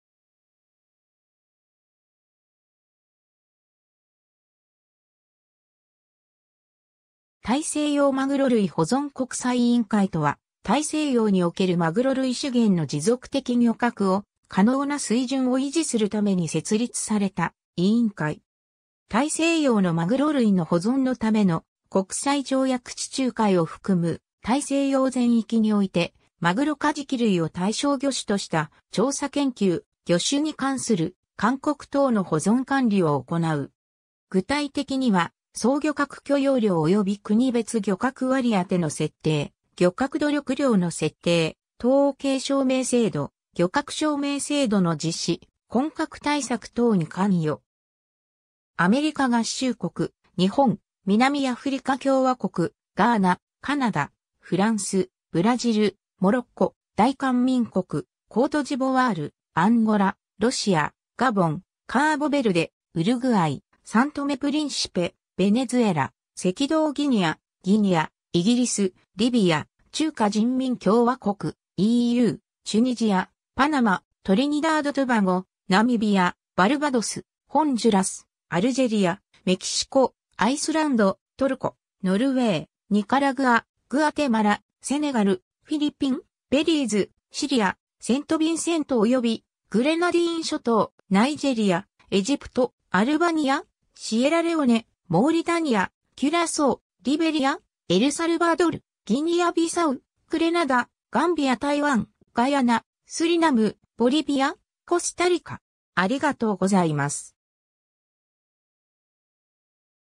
大西洋まぐろ類保存国際委員会
大西洋まぐろ類保存国際委員会（たいせいようまぐろるいほぞんこくさいいいんかい、英語: International Commission for the Conservation of Atlantic Tunas, ICCAT）とは、大西洋におけるマグロ類資源の持続的漁獲を可能な水準を維持するために設立された委員会。
 大西洋のマグロ類の保存のための国際条約（日本の署名: 1966年5月14日, 批准: 1967年8月24日, 発効: 1969年3月21日） 地中海を含む大西洋全域においてマグロ・カジキ類を対象魚種とした調査研究、魚種に関する勧告等の保存管理を行う。
具体的には、総漁獲許容量及び国別漁獲割当の設定、漁獲努力量の設定、統計証明制度、漁獲証明制度の実施、混獲対策等に関与。